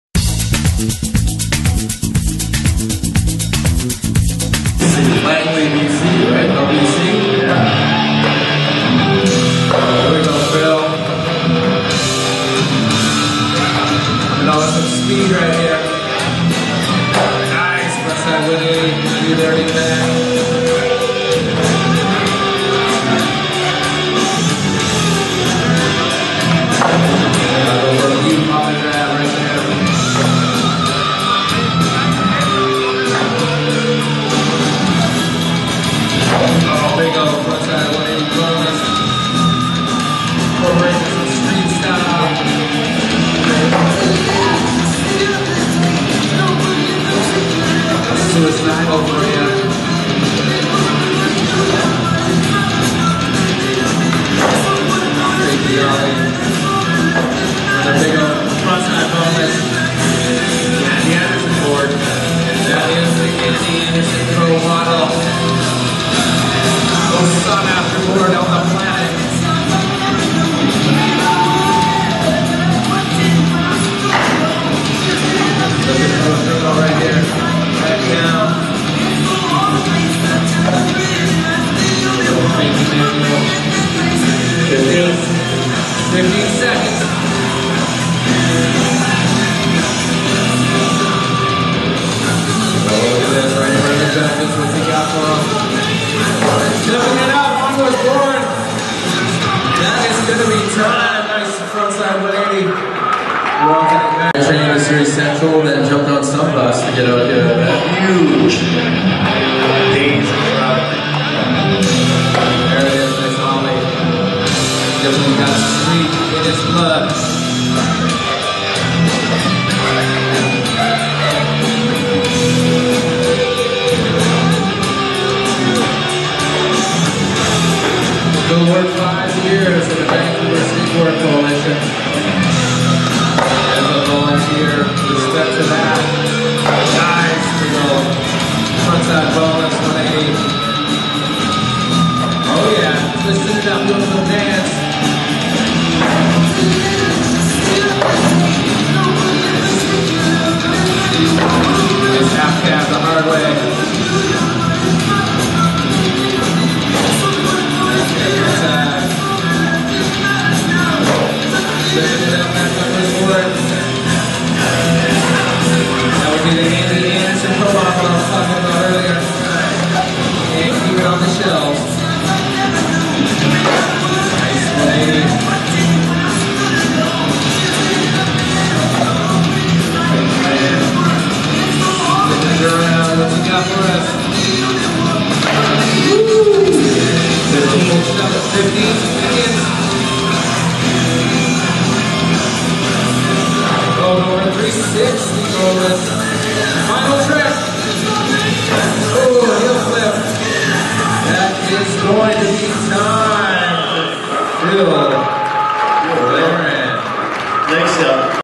Phil Larin - 13th Place Am World Round-Up 2019
Phil's first and second runs in the finals at the World Round-Up Freestyle Skateboard Championships 2019